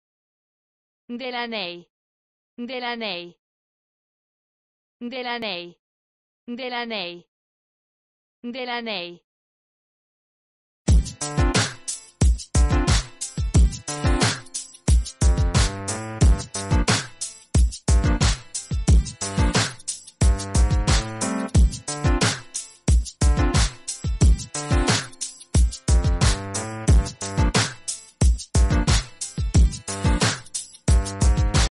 How do you say 'Delaney' in Spanish?
How to say 'Delaney' in Spanish? Listen to hear the pronunciation.